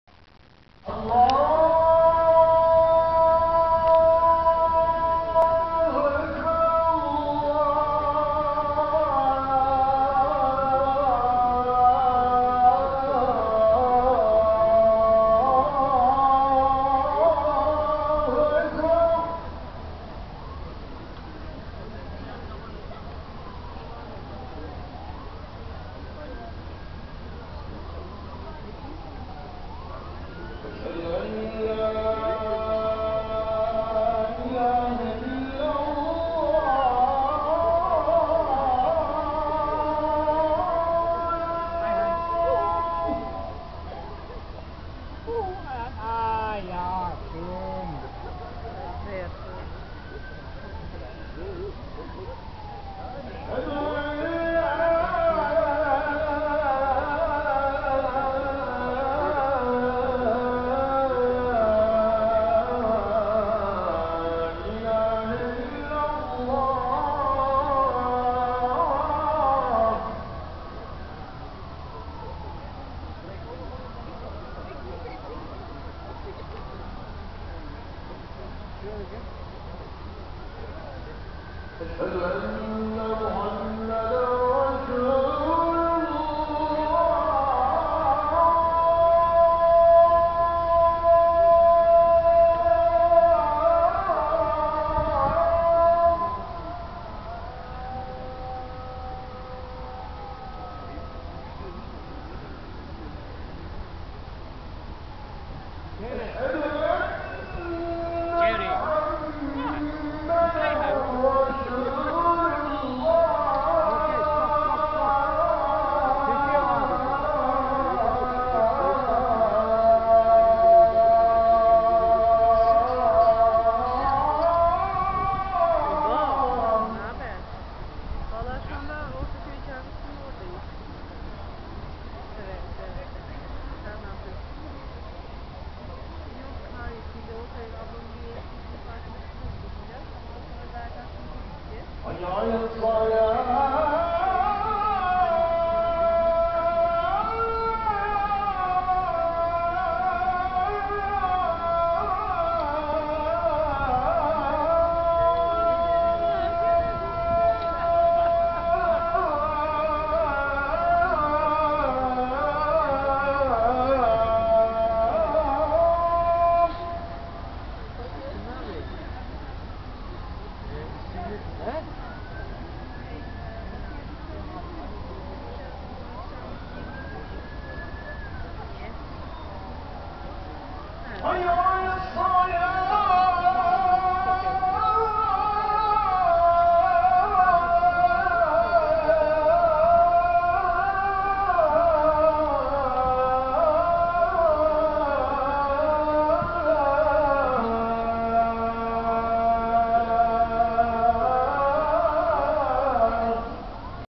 Isztambul, imára hívás az Ortaköy dzsáminál
Ortaköy Isztambul egyik leghangulatosabb kerülete. Az Ortaköy dzsámi, a Boszporusz híd és a Boszporusz látványának különleges elegye. Itt mindig sokan vannak, különleges atmoszférájú hely. Az ezán-t (imára hívó ének) itt egyszer mindenképpen érdemes meghallgatni.